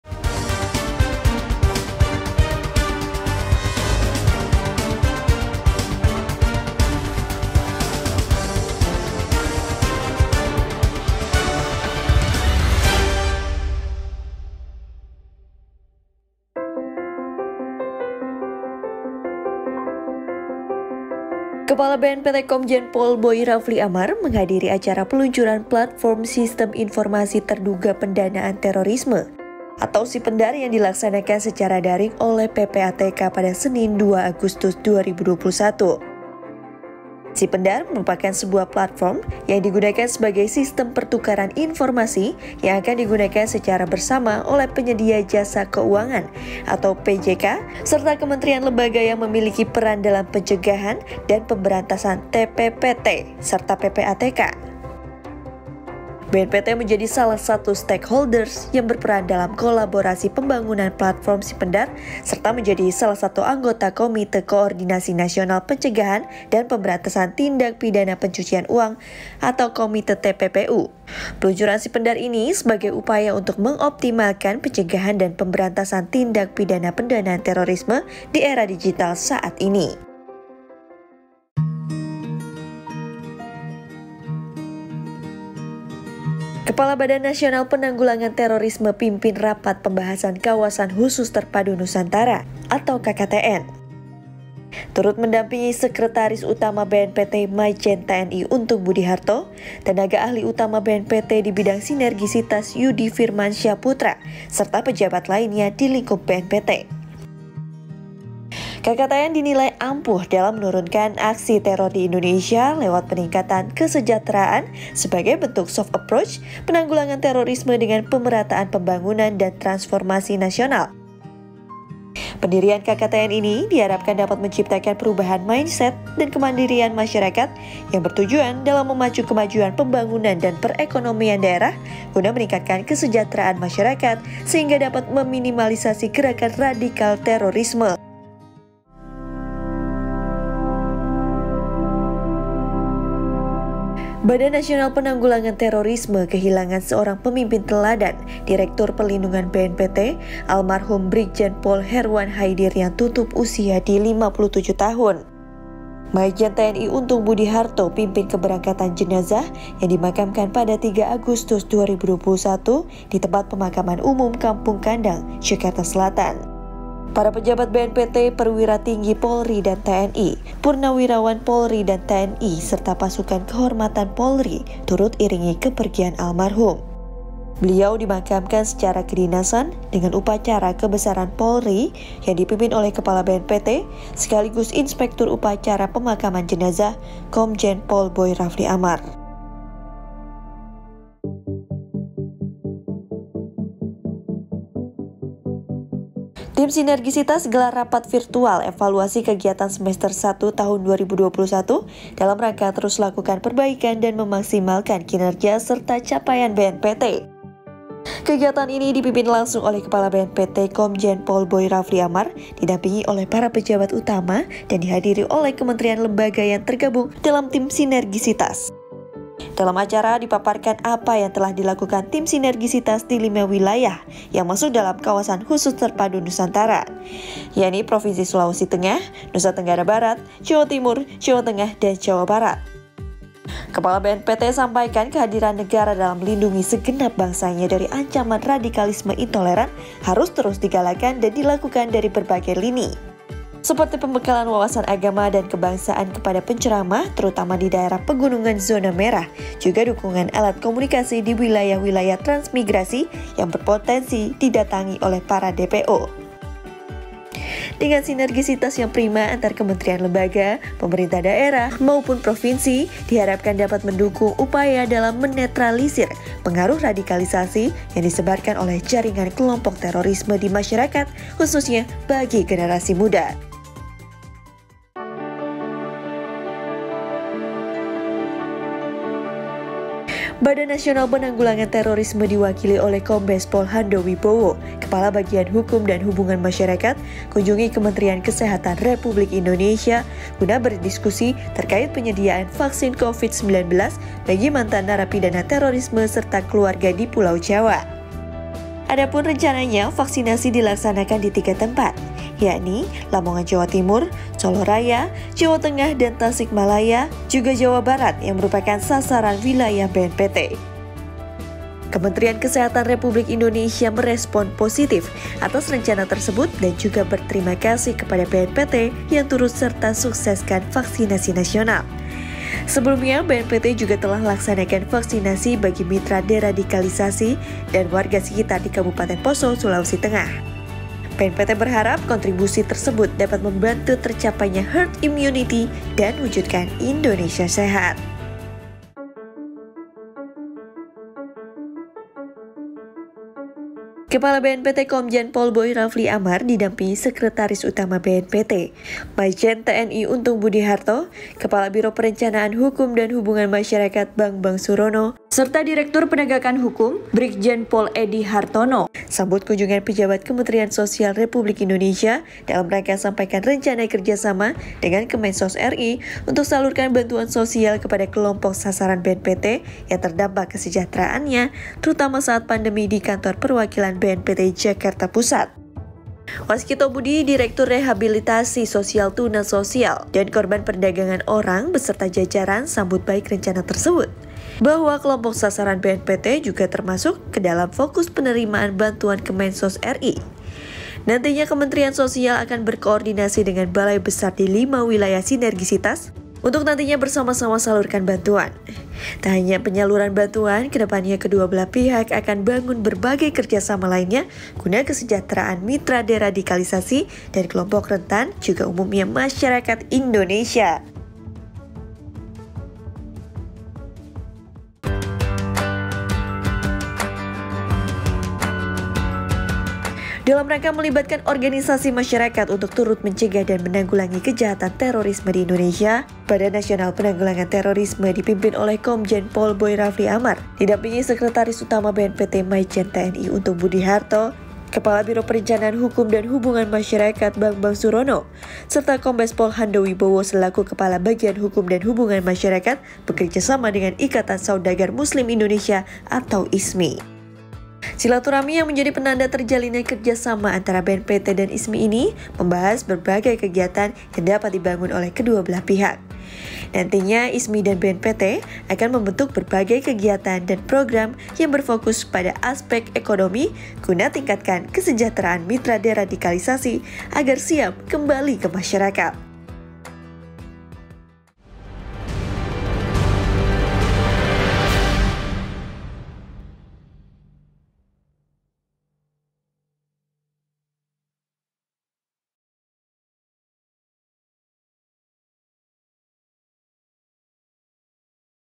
JURNAL SEPEKAN BNPT MINGGU PERTAMA AGUSTUS 2021
Video highlight kegiatan Badan Nasional Penanggulangan Terorisme selama sepekan